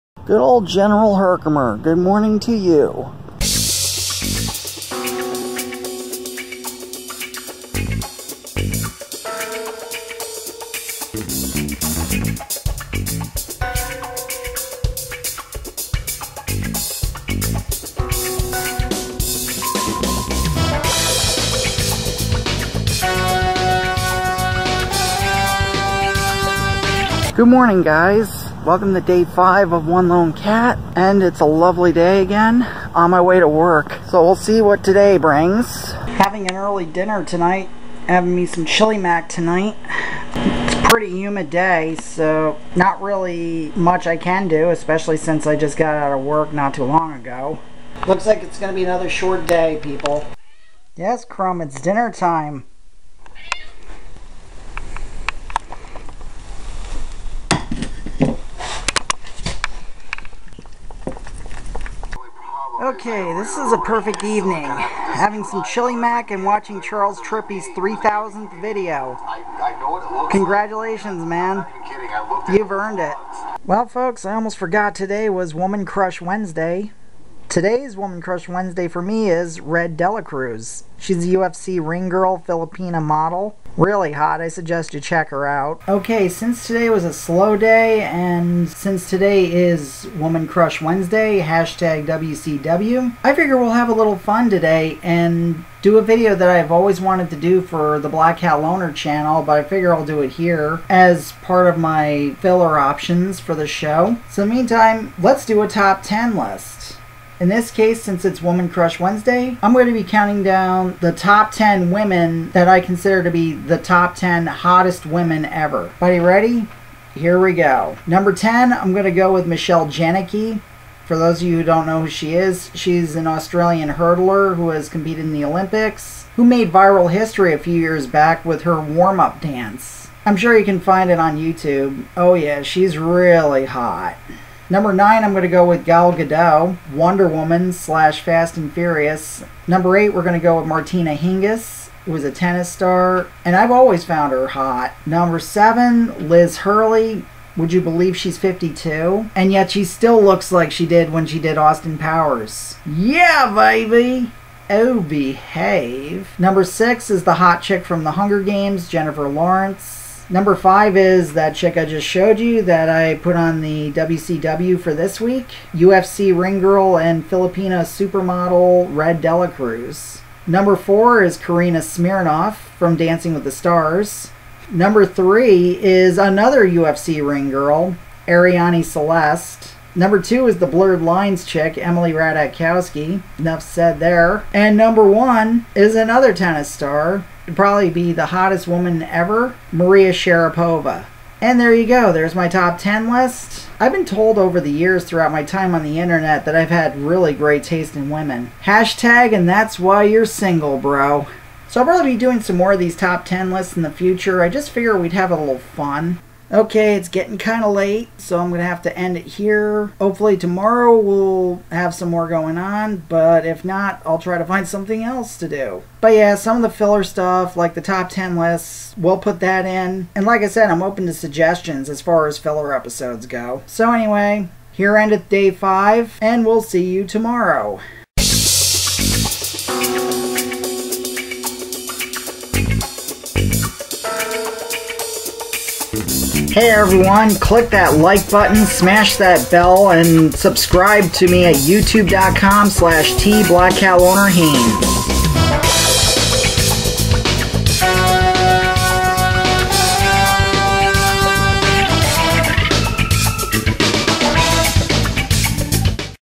THE TOP 10 WCW HOTTEST WOMEN (#5- 7/19/17)
In honor of Wednesday being Woman Crush Wednesday, I decided to count down my top 10 hottest women.